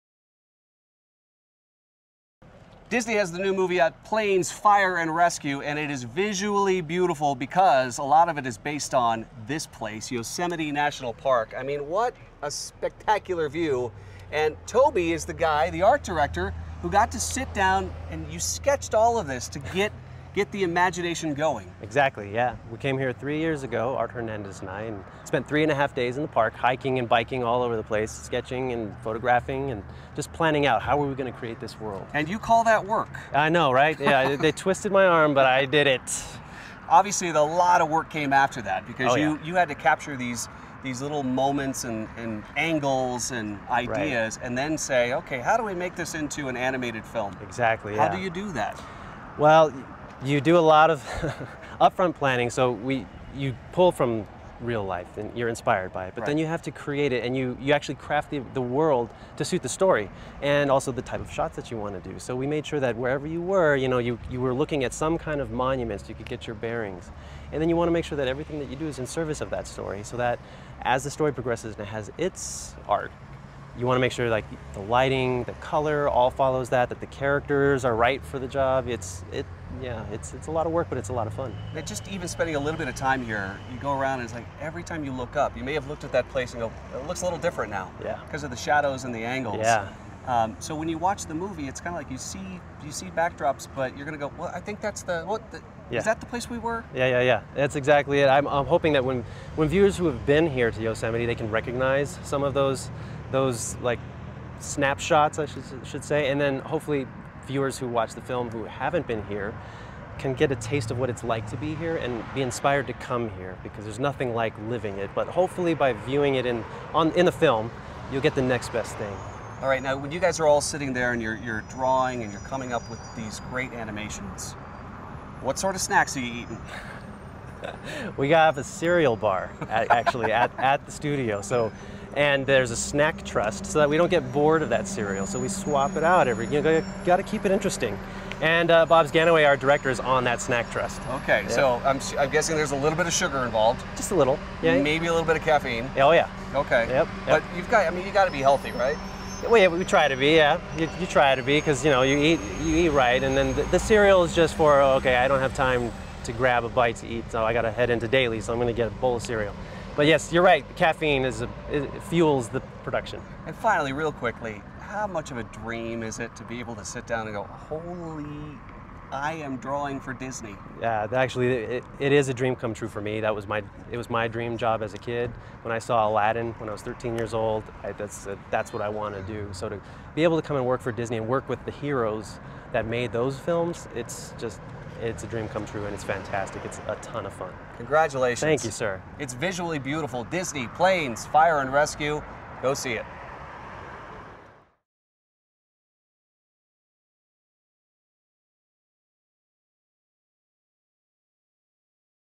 Disney PLANES: FIRE & RESCUE | Art Director, Toby Wilson | Behind The Scenes with Scott Carty
Ever dreamed of being an Art Director for Disney? Toby Wilson did...and now he's living his dream as Art Director of PLANES: FIRE & RESCUE. Had a great visit with him at Yosemite National Park.